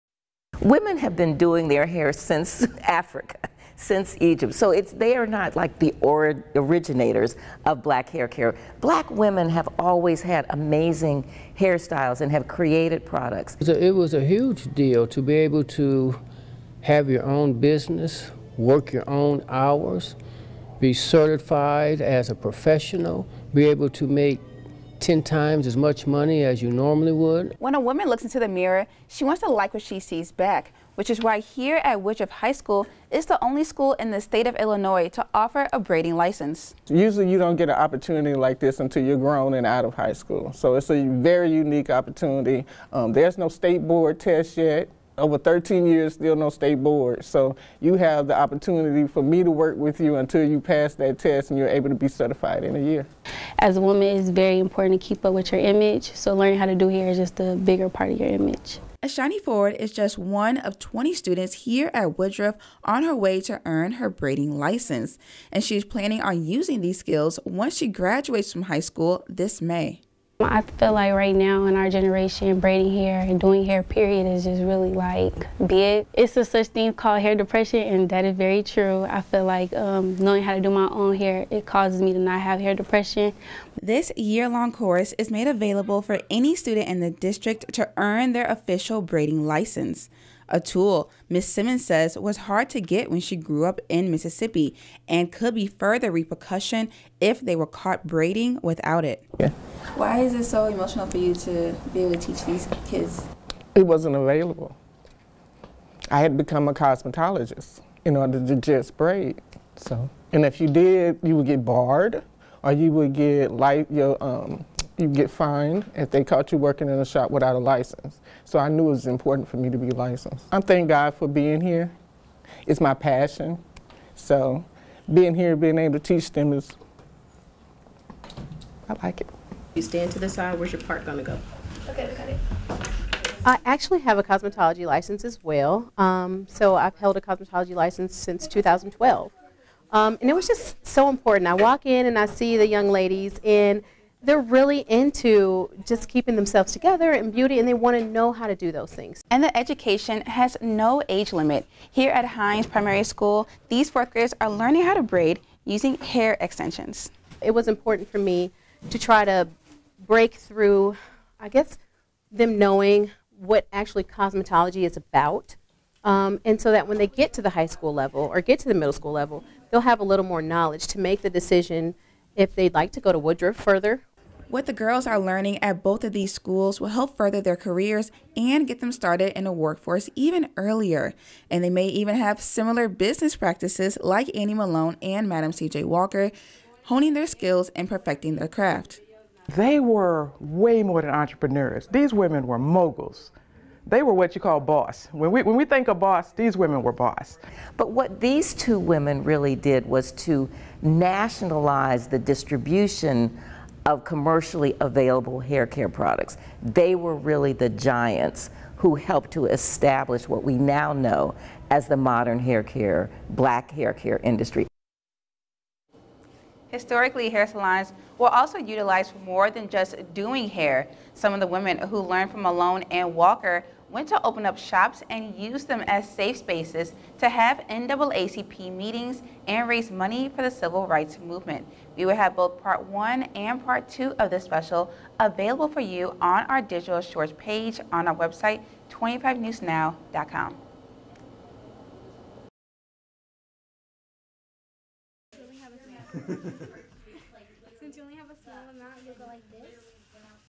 Annie vs Madam CJ Walker
Both pioneering women help to lay a foundation which encourage women to make their own money helping to beautify women.